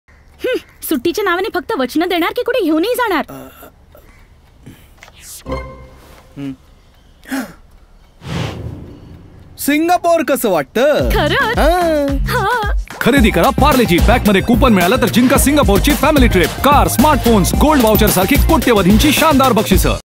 Parle-G Offer TVC - Trip To Singapore | Marathi
Start packing for Singapore! Buy any pack of Parle-G up to Rs. 5 and stand a chance to get lucky!
If you find a coupon inside the pack, you can win prizes such as a family trip to Singapore, car, smartphone, gold vouchers and lots more.
T&C apply.